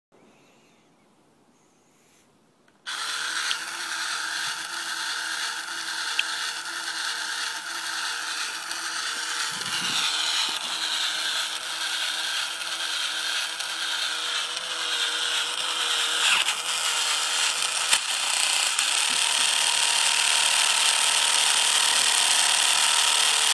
Rowing Man catches a crab
A wind-up rowing whatchamacallit catches a crab.